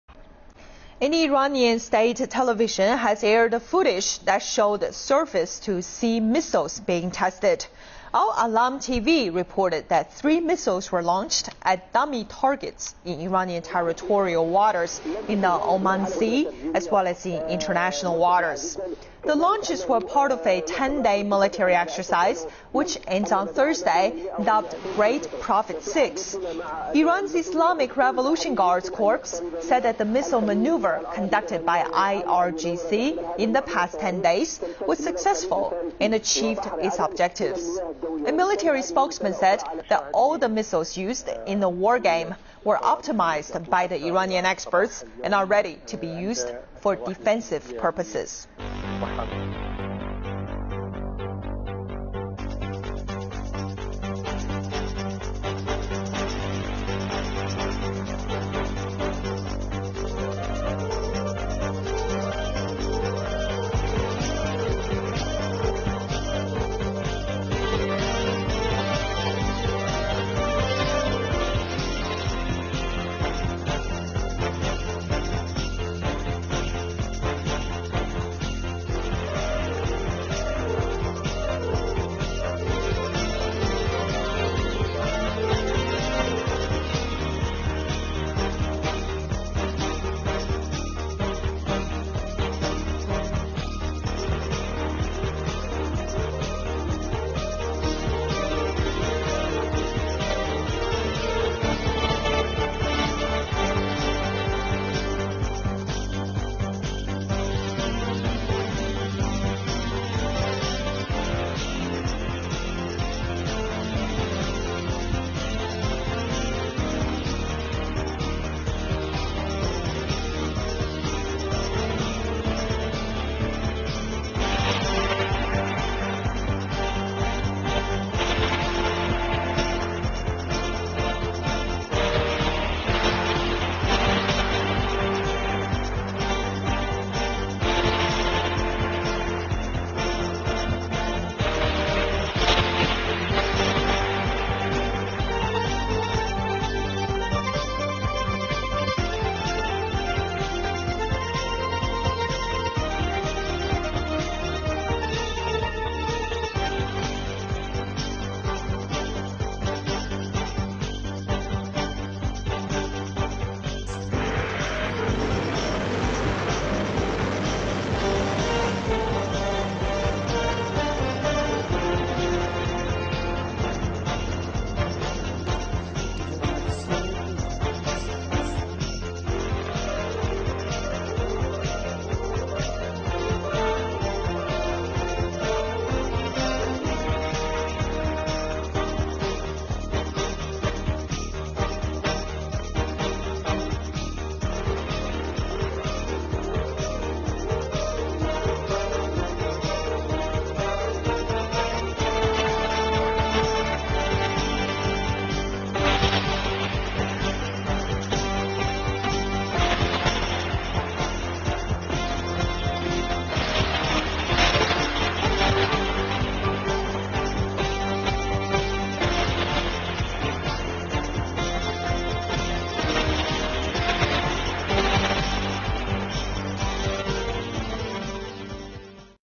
Iranian Military Domestic Technology, Surface to sea missiles, S 300, hidden silos Shahab 3
Iran's upgraded S 200 the equivalent of S 300 and all domestically made air defence technology for defence purposes.